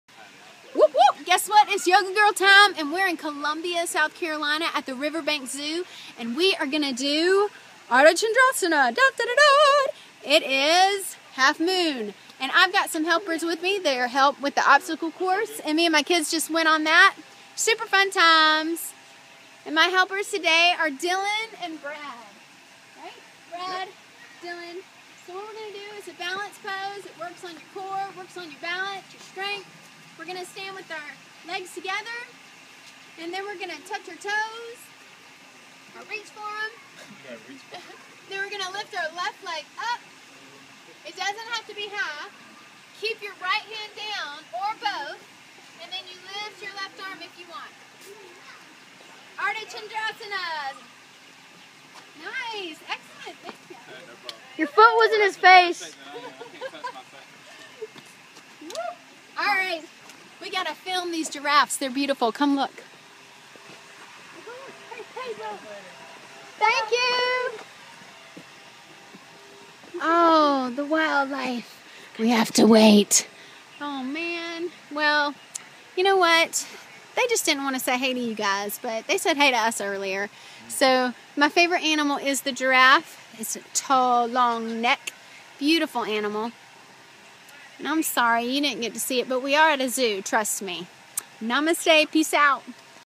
yoga zoology
Riverbanks best employees: Brad & Dillion! Trying out Ardha Chandrasana/half moon.